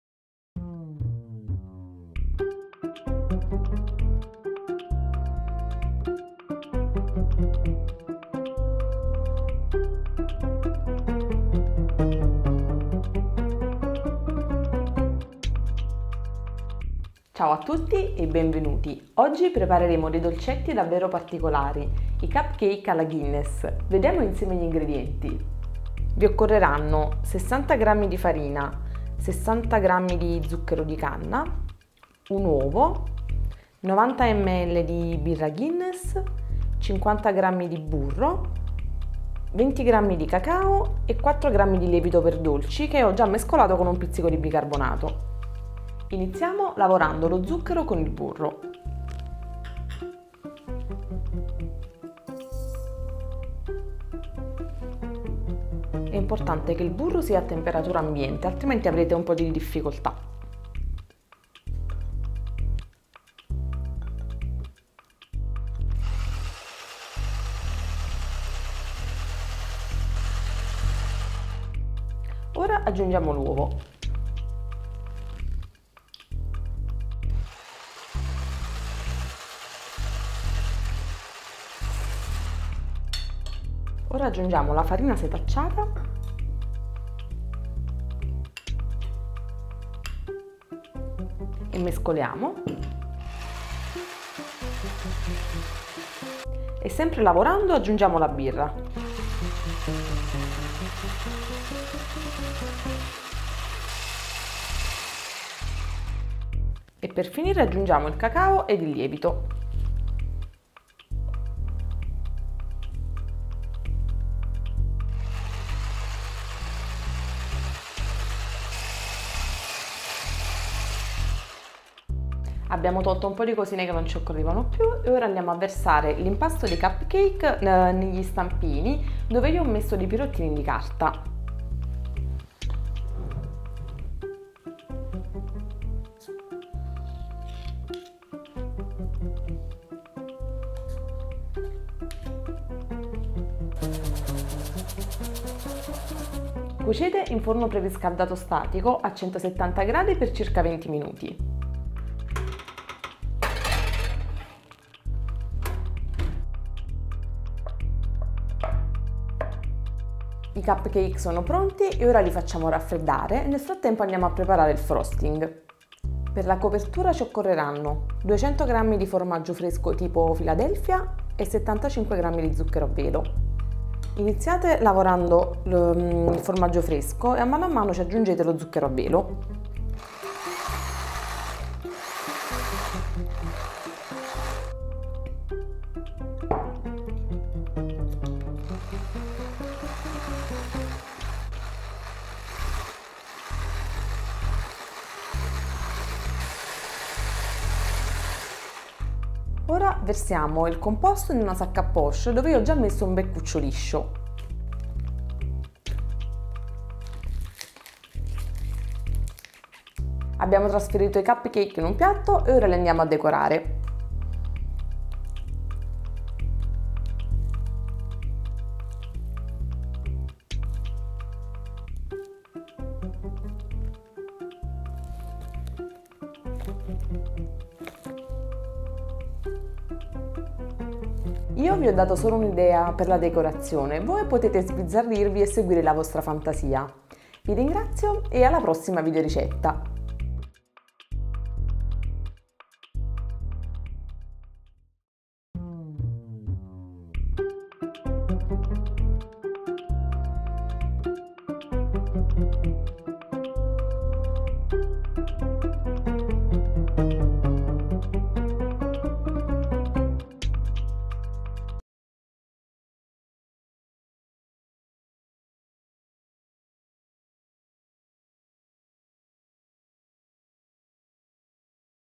Cupcake alla Guinness - Gnam Gnam
Cupcake cioccolatosi e con una soffice copertura! Provali e mi dirai! :)